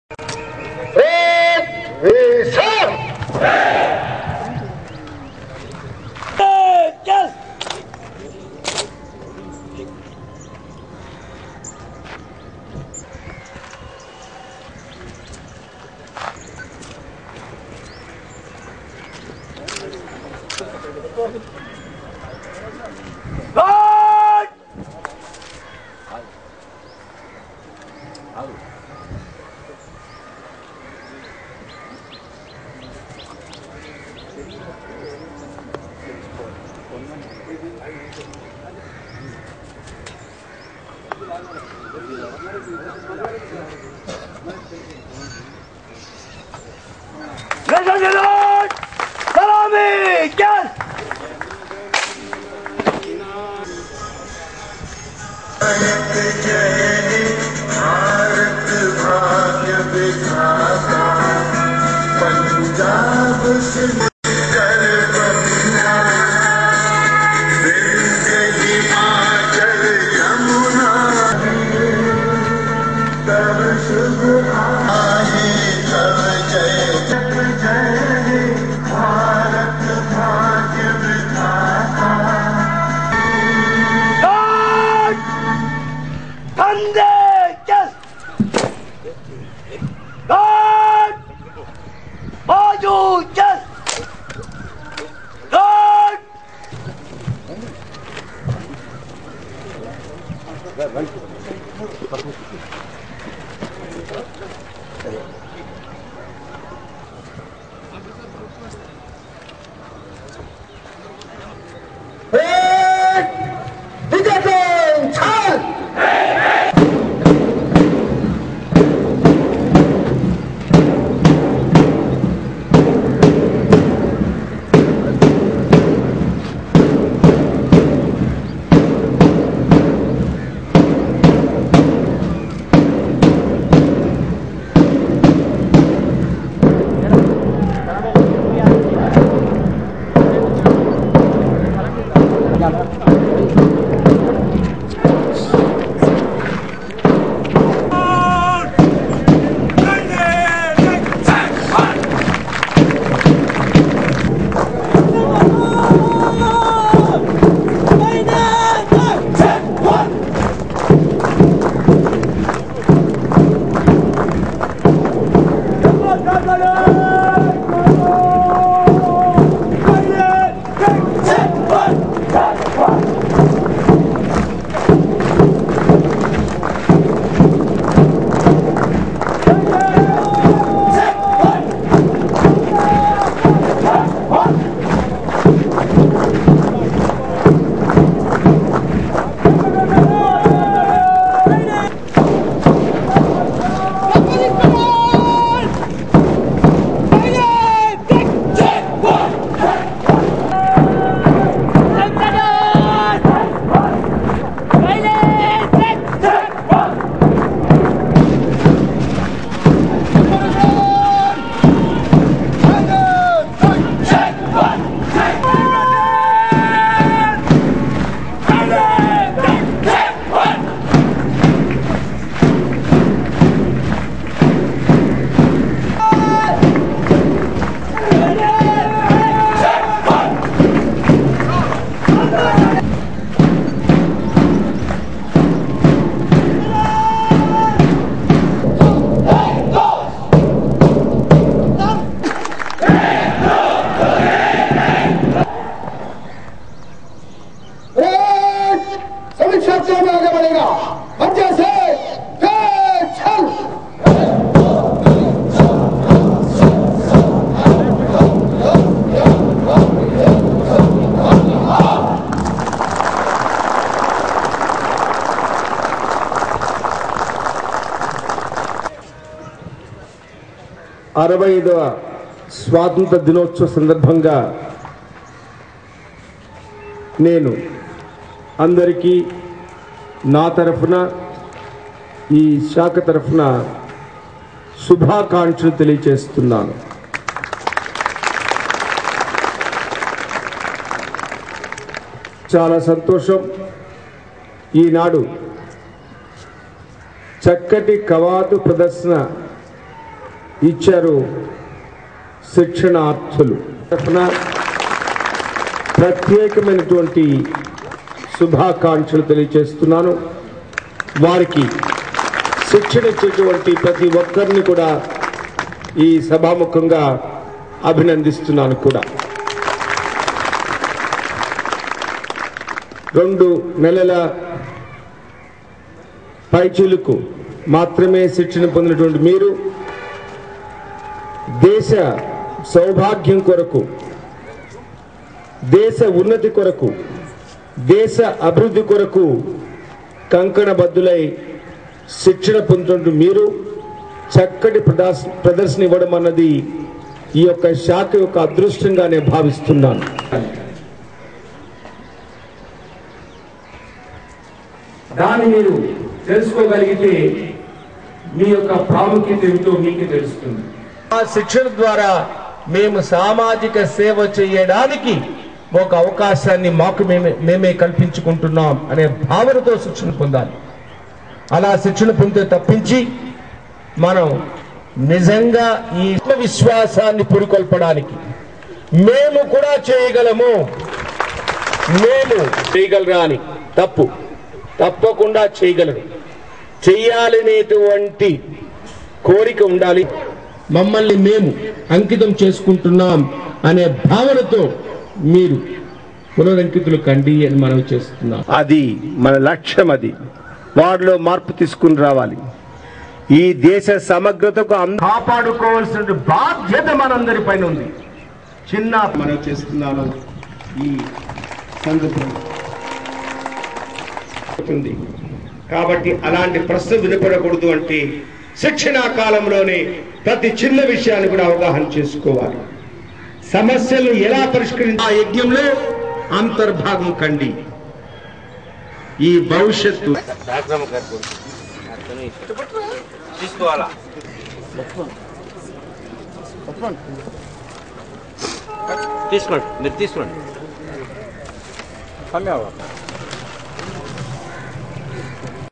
The Hyderabad Deccan : Parade organized at Central Prison Chanchalguda Hyderabad.
The Hyderabad Deccan : Parade organized at Central Prison Chanchalguda Hyderabad. Rayees Azam.